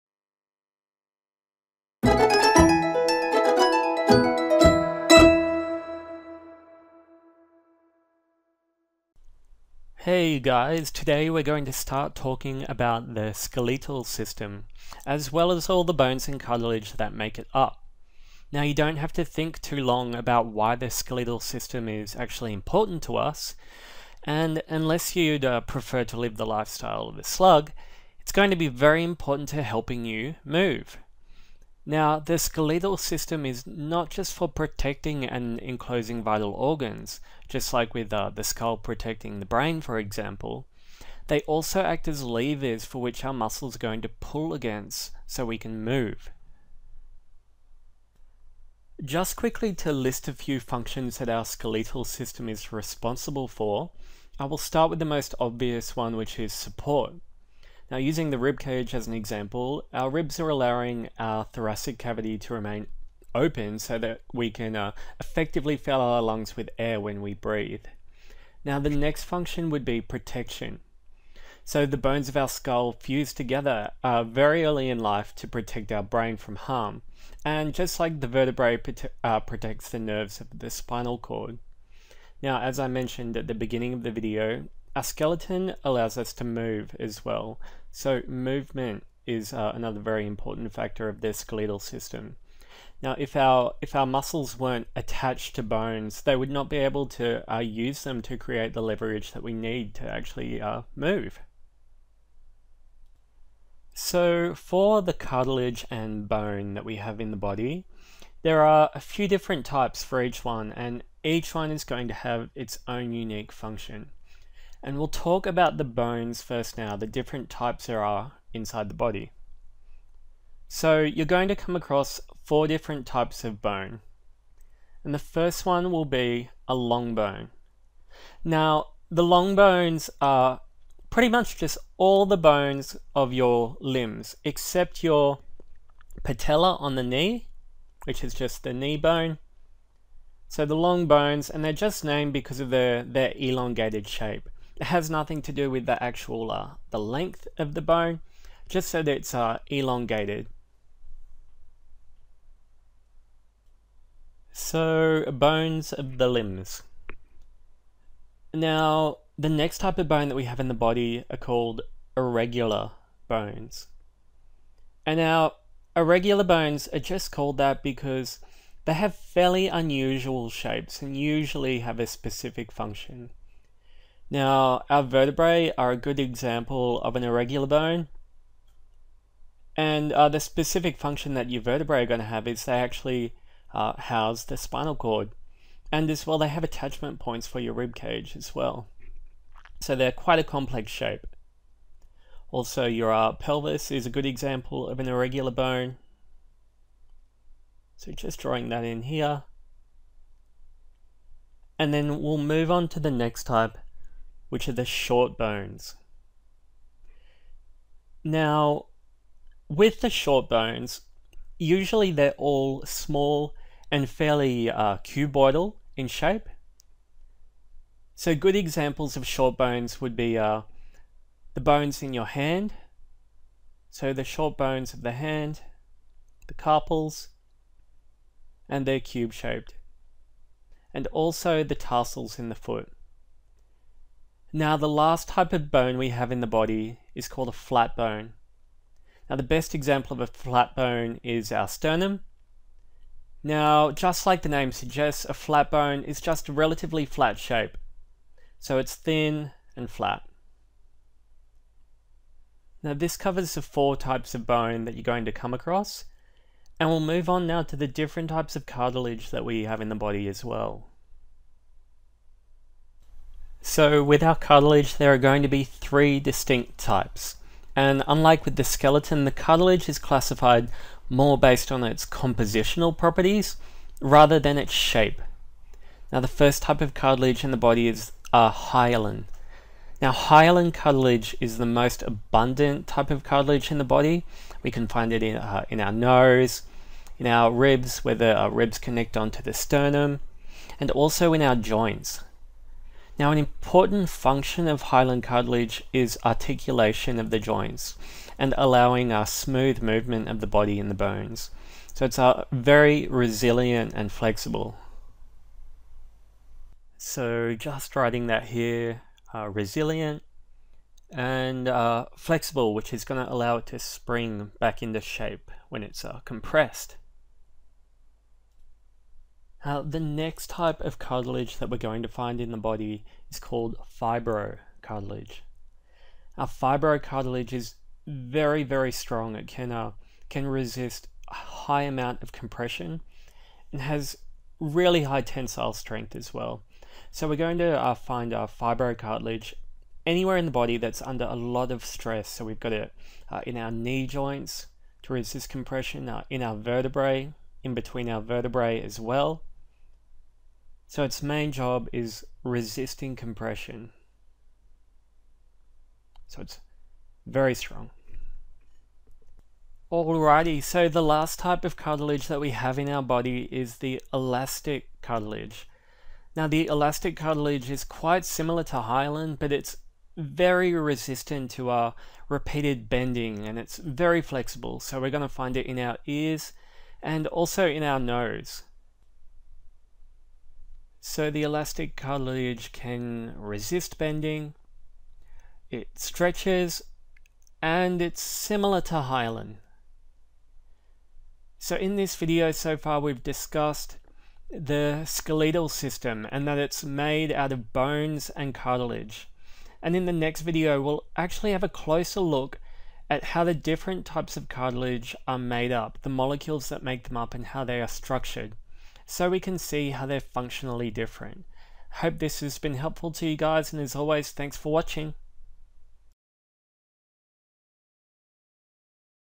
Skeletal System, Bones and Cartilage
This introduction to the skeletal system briefly discusses the components that make up this system, the bones and cartilage. We also briefly talk about the way in which this skeleton is beneficial to us for life!

Video Number- Anatomy 1 -07